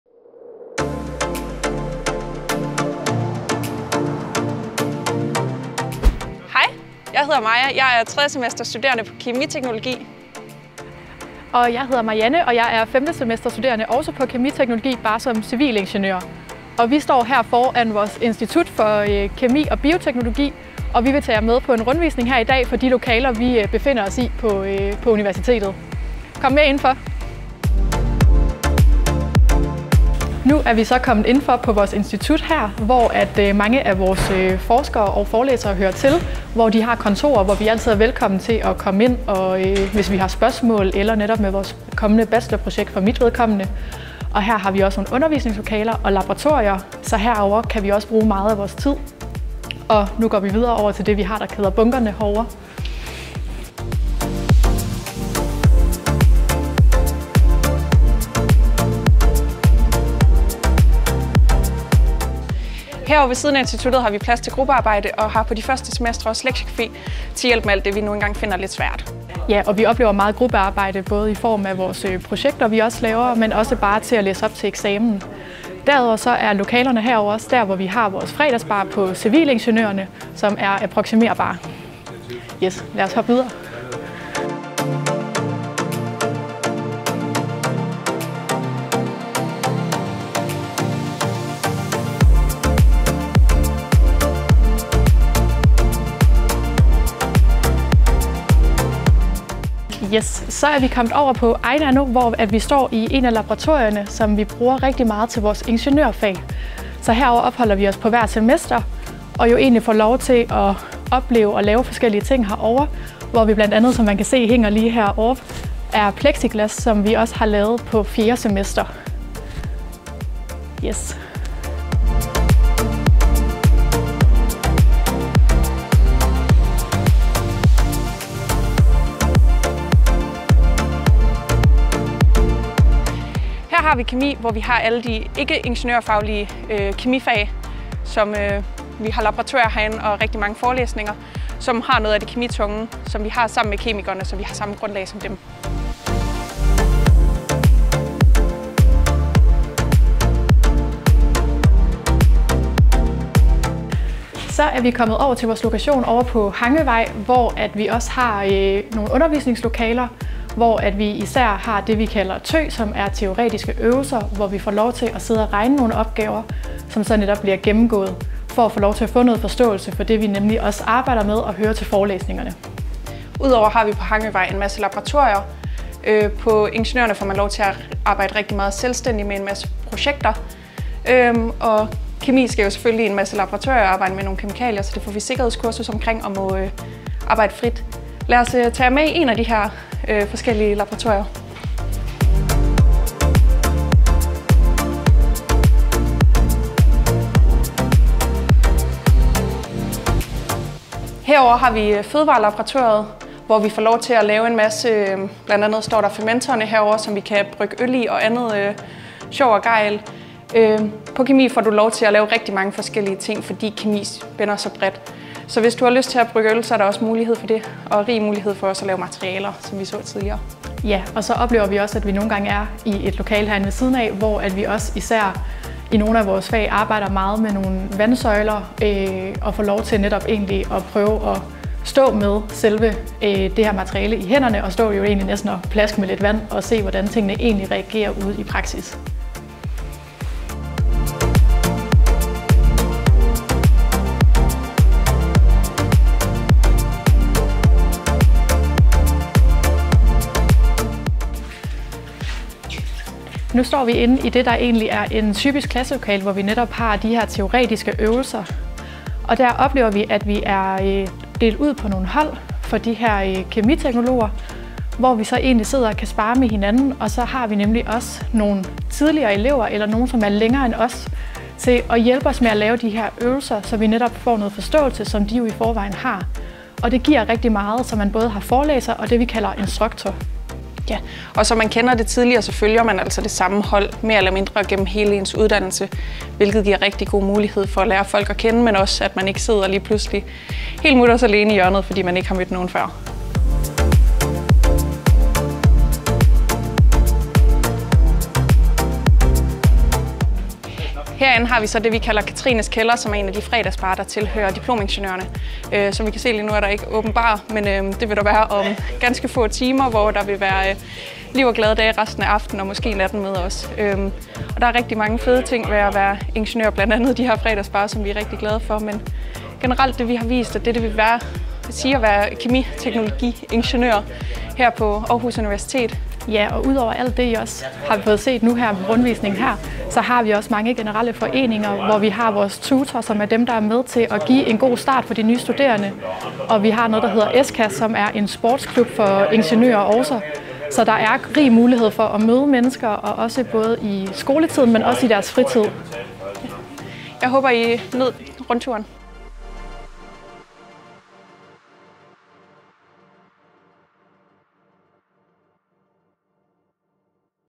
Virtuel rundtur på Kemiteknologi
Kom med indenfor dørene på ingeniøruddannelsen i Kemiteknologi på Aarhus Universitet. Her viser vores studerende rundt i de bygninger, man kommer til at have undervisning og øvelser og projektarbejde i, hvis man læser til Kemiteknologi. Følg med og læs mere på ingeniør.au.dk.